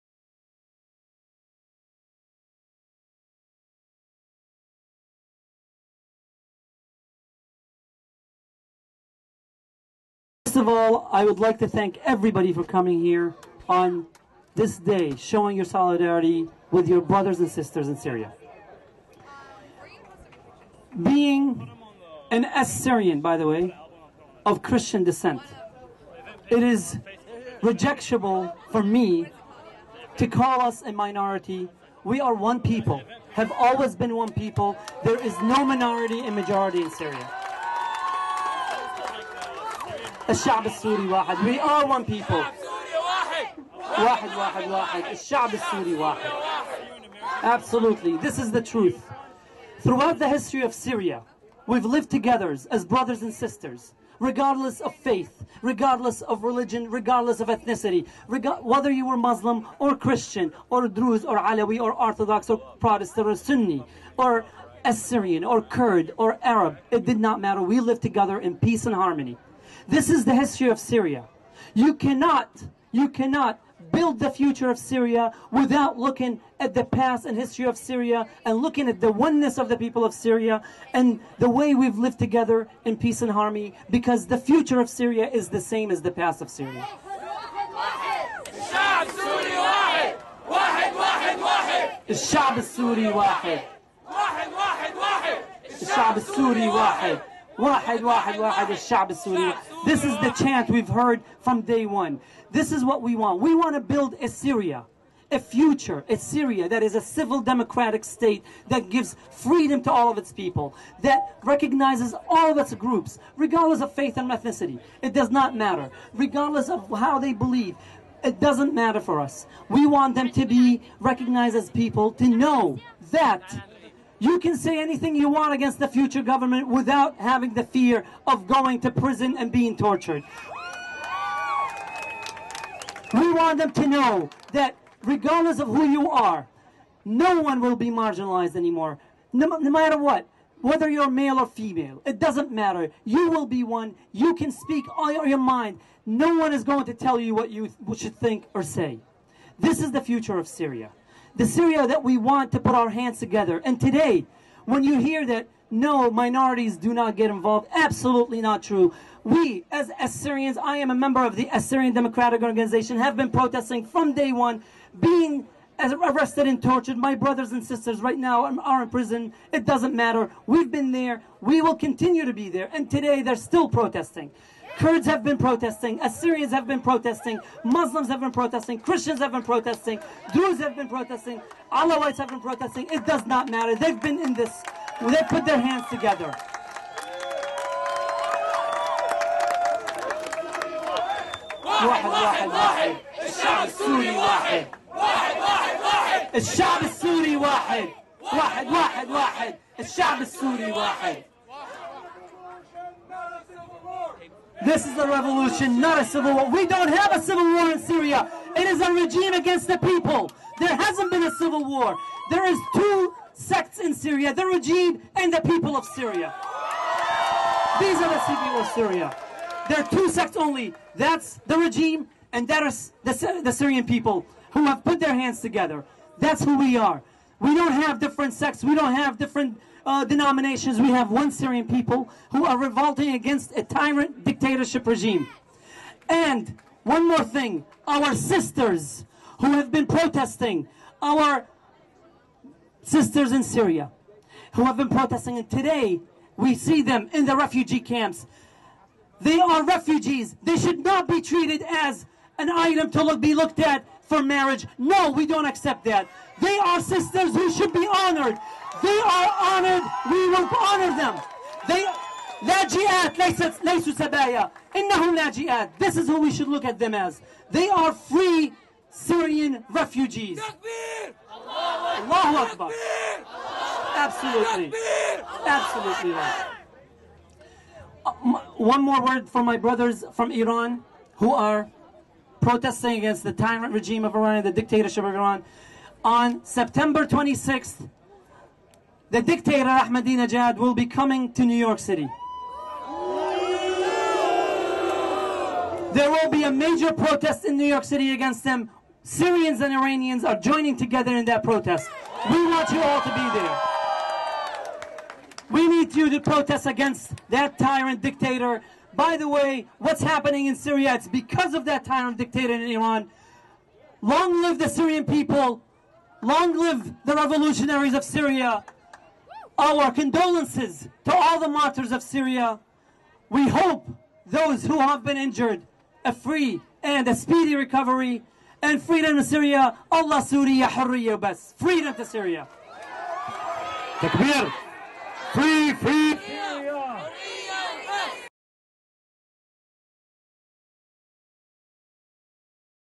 George Stifo Syrian Christian Freedom Rally Washington DC
George Stifo, a Christian Syrian, addresses the crowd on the White House lawn in Washington DC.